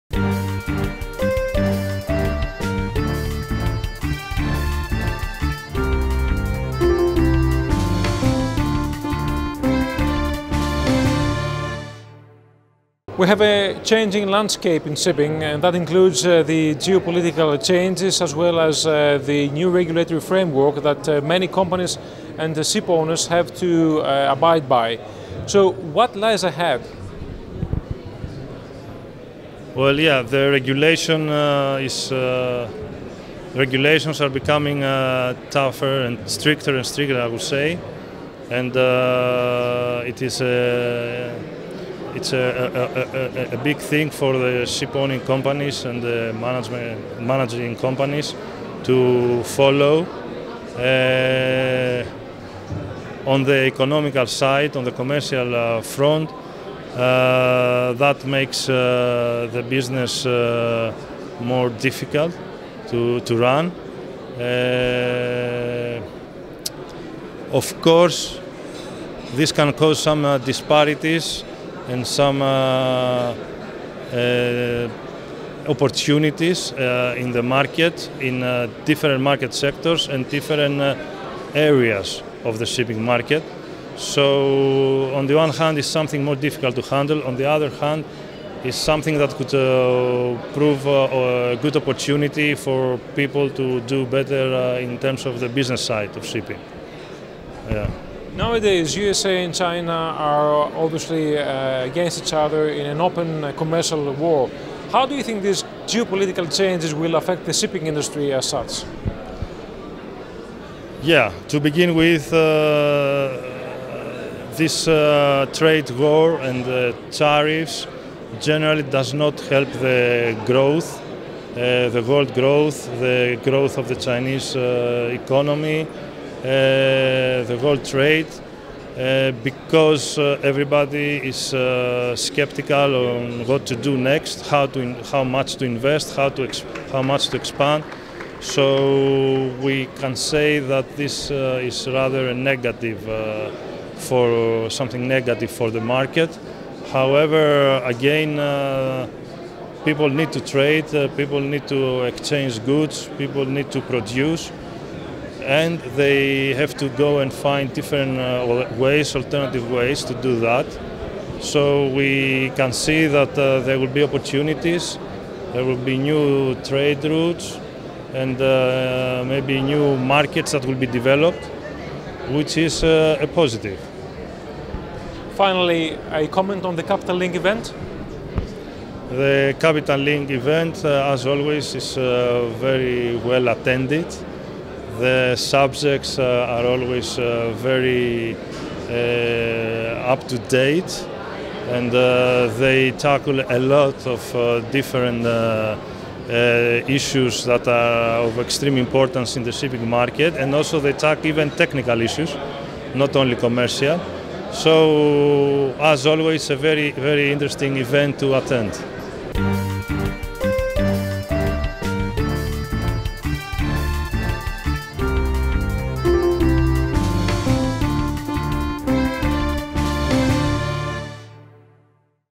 2019 9th Annual Operational Excellence in Shipping - Savvas Athanasiadis Interview
Mr. Savvas Athanasiadis, Managing Director - Clarksons Platou (Hellas) Ltd